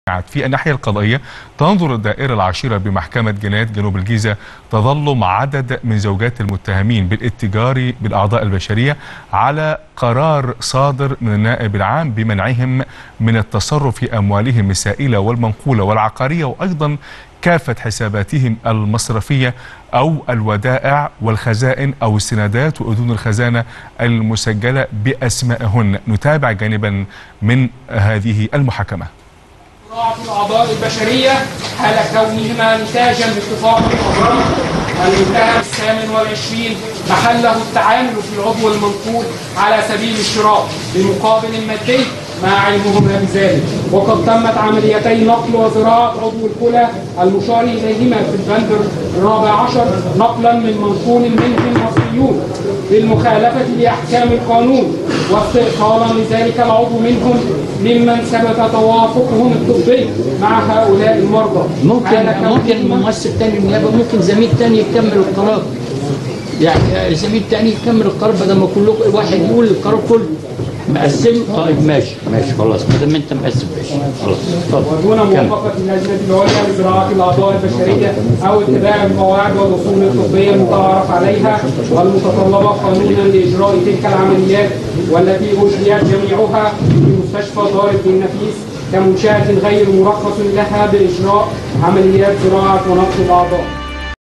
جانب من جلسة نظر تظلم زوجات متهمي الاتجار بالأعضاء البشرية على منعهن من التصرف بأموالهن
الموقع الرسمي للقناة  ..